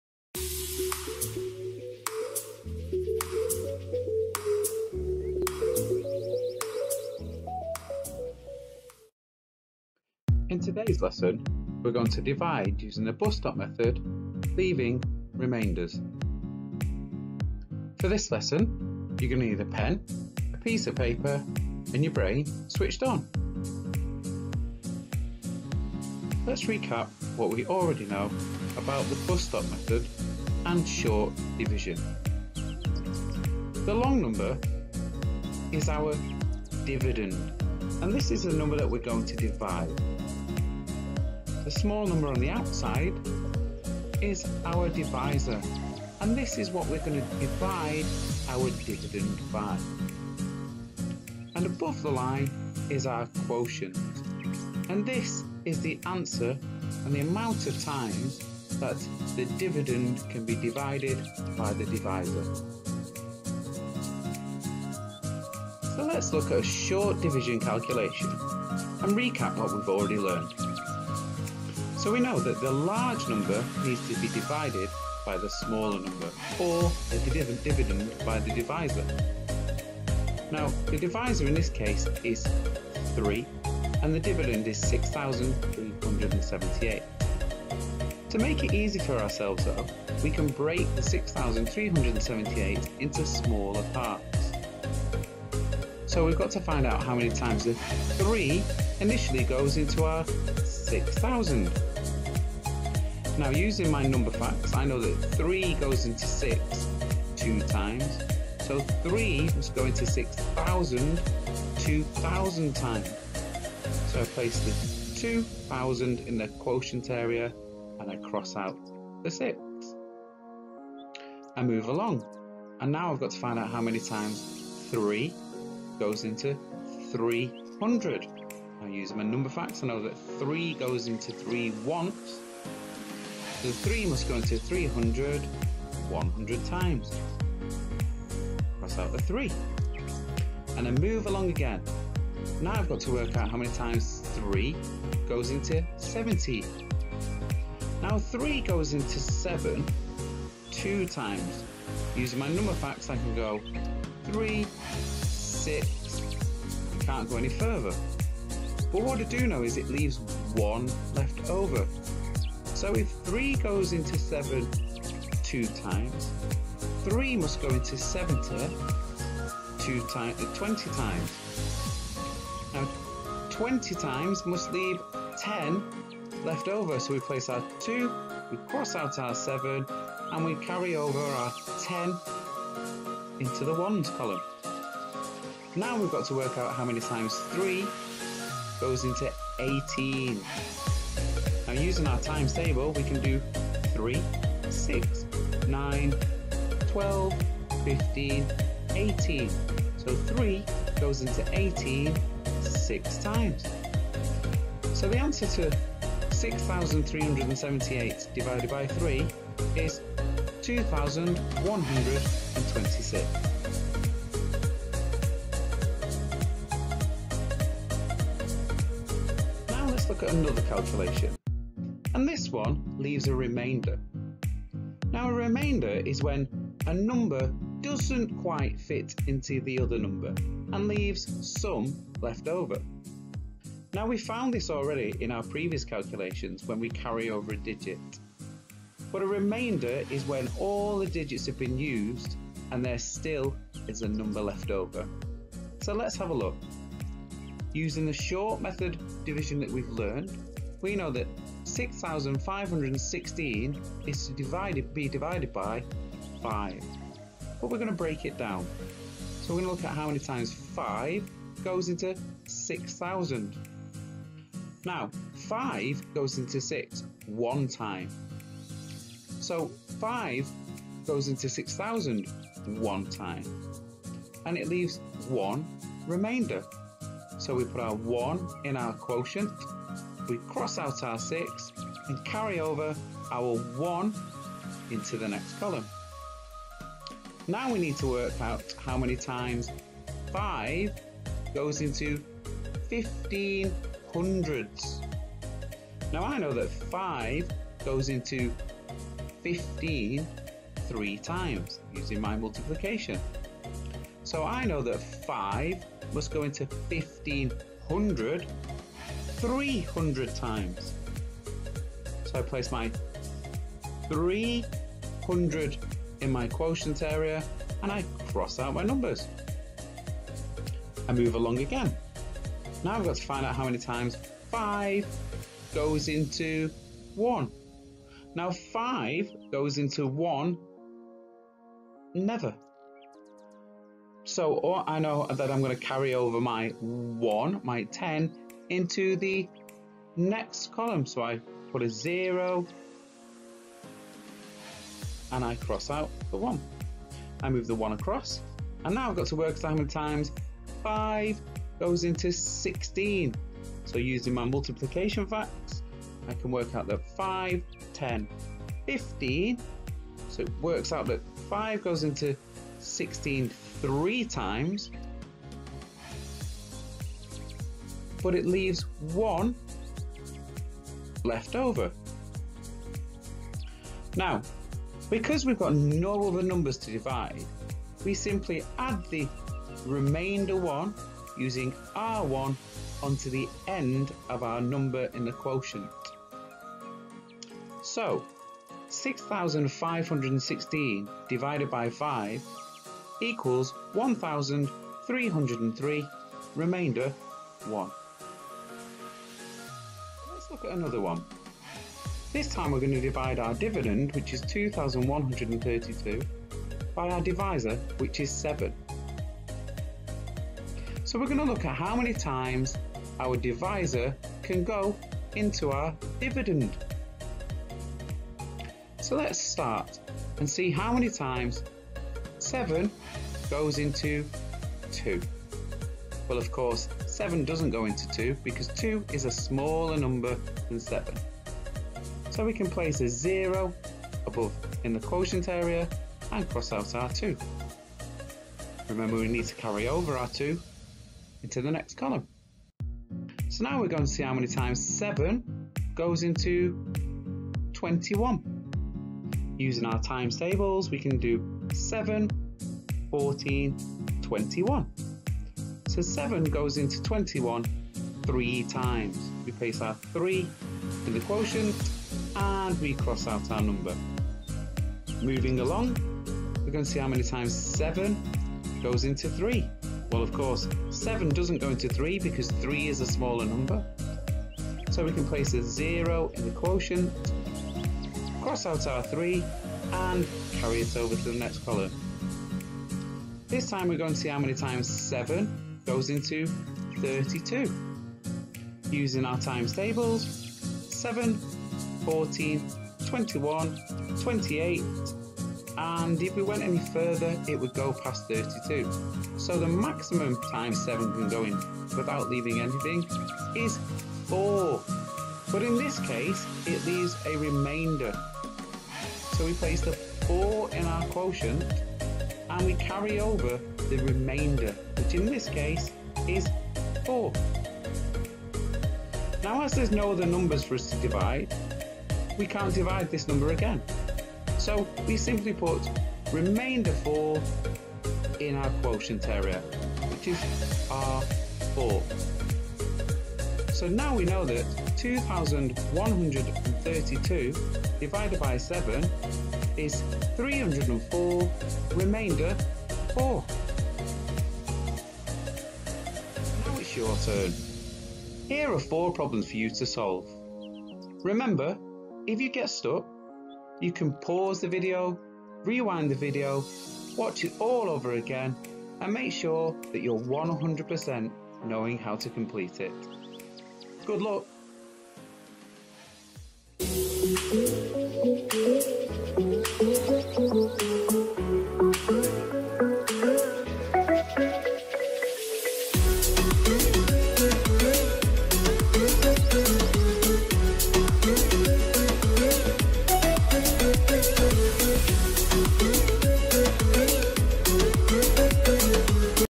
Bus stop method to divide (with remainders) New for 2018
Learn how to use the bus stop method to divide leaving remainders.